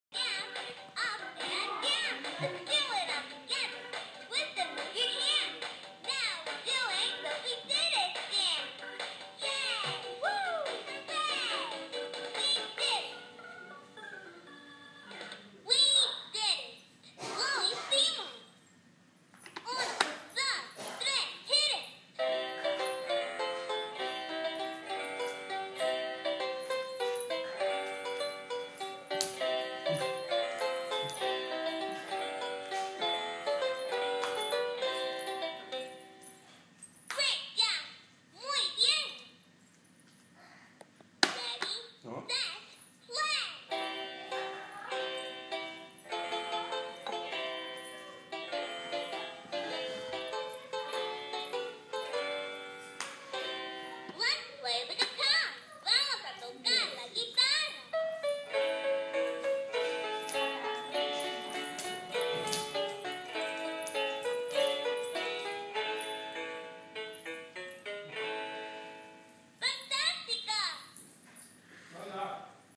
DEC-13-2012 Maanya's Dance For Dora Music
Maanya dances to the Dora Guitar toy music